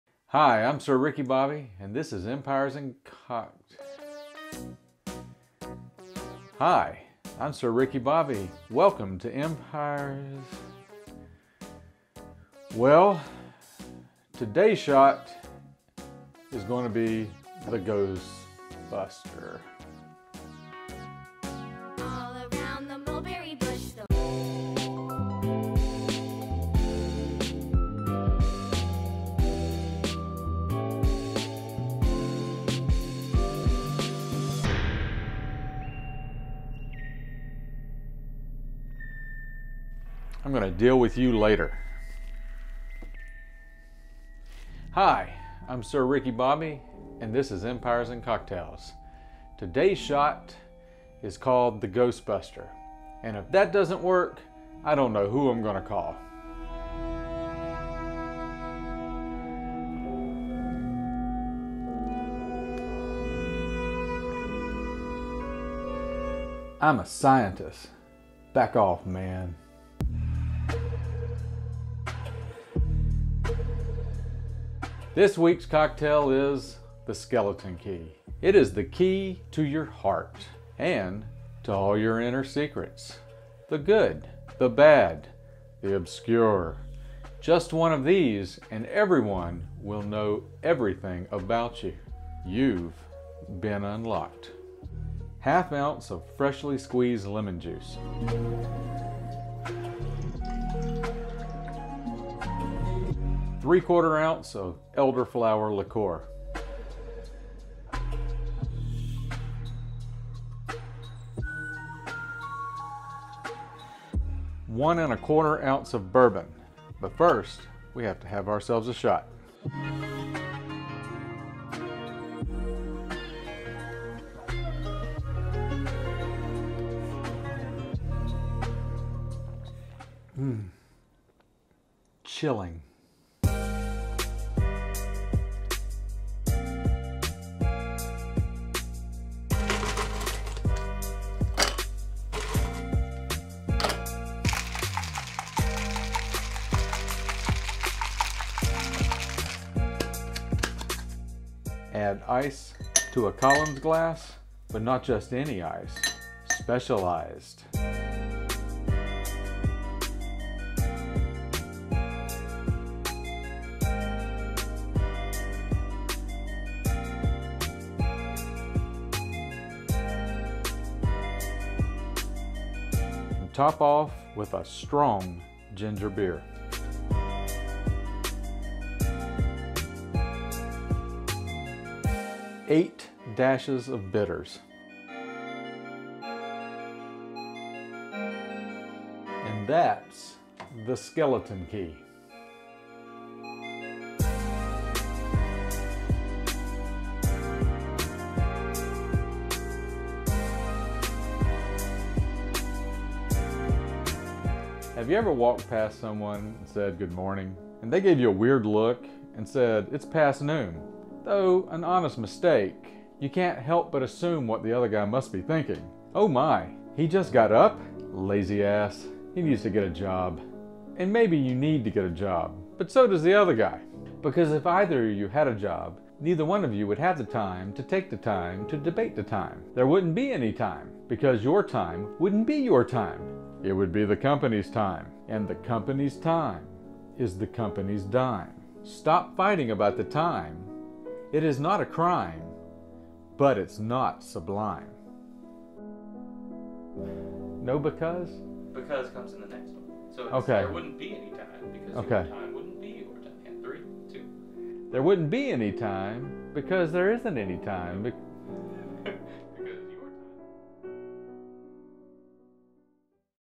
Skeleton Key {This Cocktail will Unlock your Haunted House}
Halloween is nye. Sir Ricky Bobby is running out of time to show you which potions could save your mortal soul! So, in a final attempt, a life saving throw, a last chance power drive, The Sir is pulling out all the stops and unlocking all the doors. Thank God he brought his Skeleton Key.

Recipe:

1/2 Ounce of Lemon Juice
3/4 Ounce of Elderflower Liquor
1 1/4 Ounces of Bourbon
Add Ice
Shake, Shake, Shake!!!
Strain over ice
Top of with Ginger Beer
8 Dashes of Bitters

Empires and Cocktails with Sir Ricky Bobby has been bringing you the best in Cocktail Tutorials and dim-witted humour since the Summer of 2019.